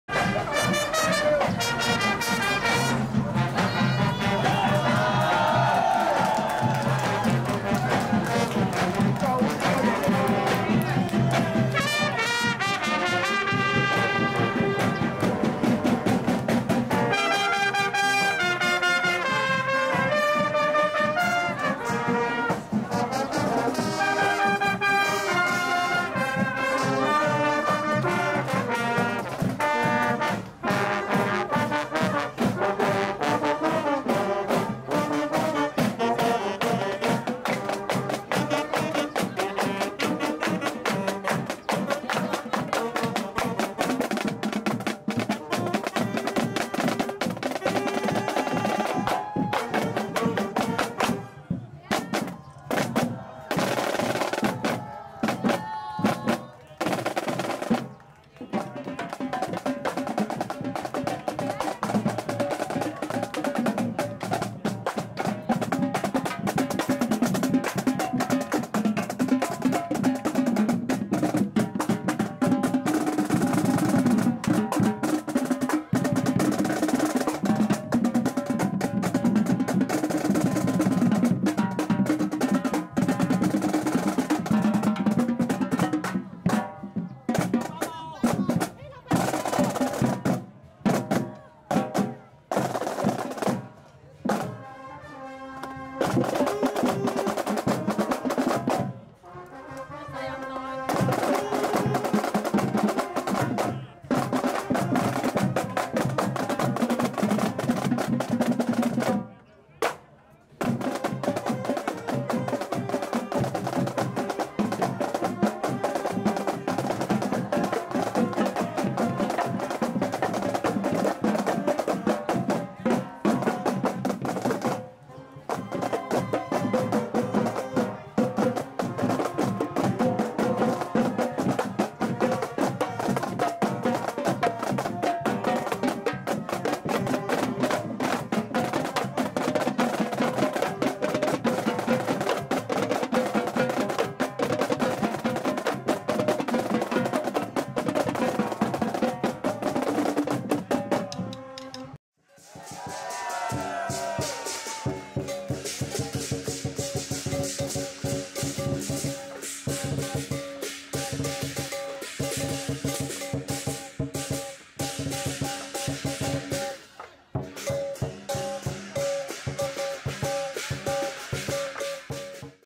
Cheng Chau Bun Festival 2017 - A Parade and Festival in Hong Kong
Hong Kong's Cheng Chau Bun Festival or as I like to call it, the Kids on a Stick Show.

General walk around shots from the sidelines. Testing and practicing using the Zhiyun Crane Stabiliser and Sony A7RII + 28mm f/2 setup